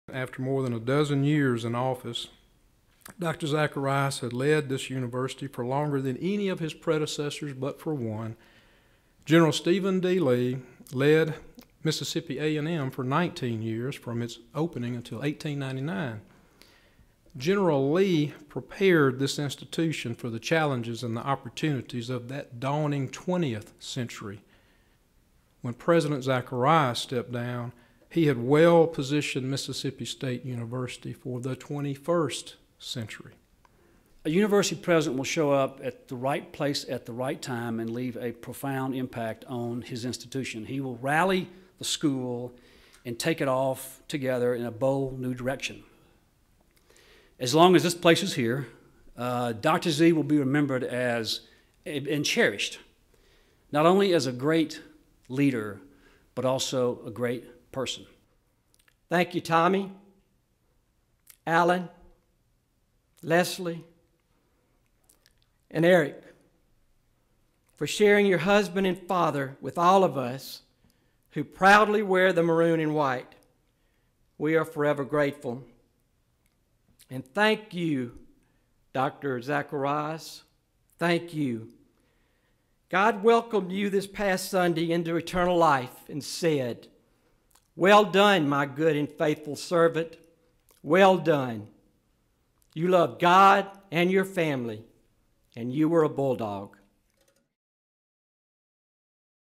Dr. Zacharias Memorial Service
MSU President Dr. Mark Keenum, best-selling author John Grisham, and Alumni Association Executive Director Dr. Jimmy Abraham speak fondly of their time with MSU President Emeritus Dr. Donald W. Zacharias during today's memorial service.